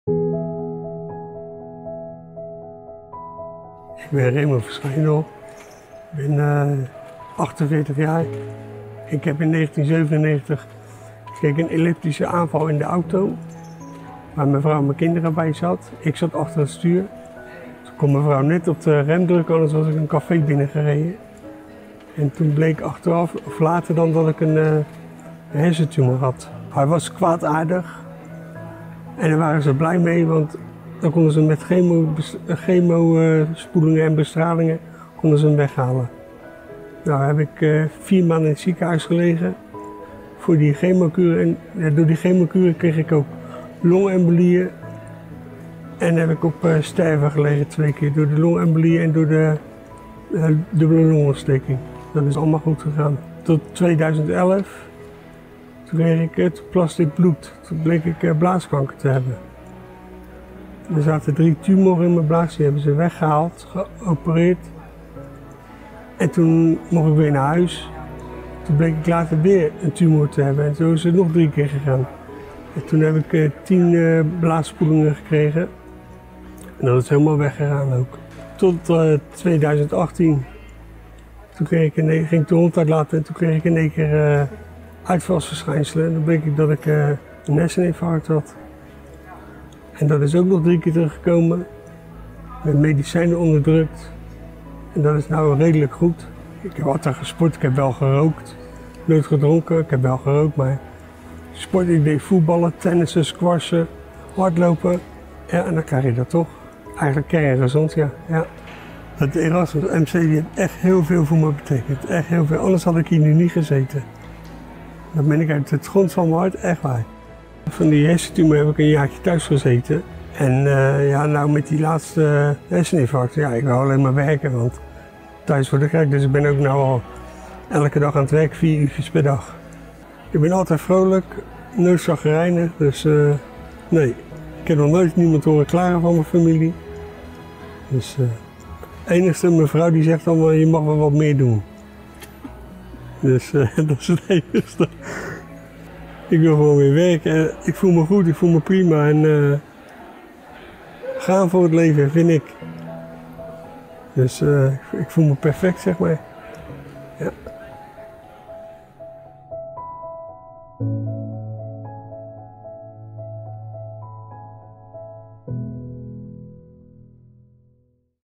Door de ogen van Raymond - Erasmus MC
Wat betekent het Erasmus MC voor jou? Wij gingen in gesprek met ex-patiënt Raymond en hij vertelt zijn verhaal.

Luister naar de Erasmus MC Podcast 'In Opname' via o.a. iTunes, Google Podcast, Deezer of Spotify